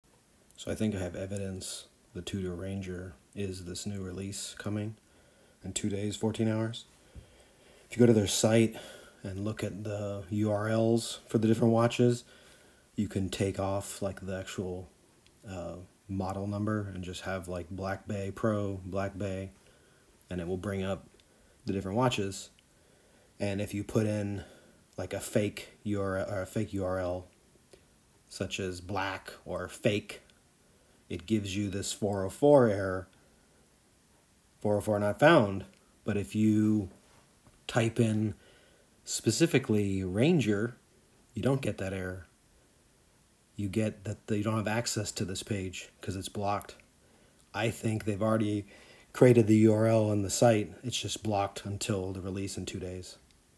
New Tudor Ranger to be dropped in 2 days! Evidence!
If you go to the URL below, you will see Tudor has this blocked. I speculate that they have created the page for their new Ranger release to commemorate the 70 year anniversary of the British North Greenland Expedition. If you put in bug URL (replace “ranger” below with “fake”), you will get a totally different “404 Not Found” error. This may be evidence that the rumors were all correct about a new Ranger coming this month!

Do you think Tudor’s July 2022 release is a new Tudor Ranger?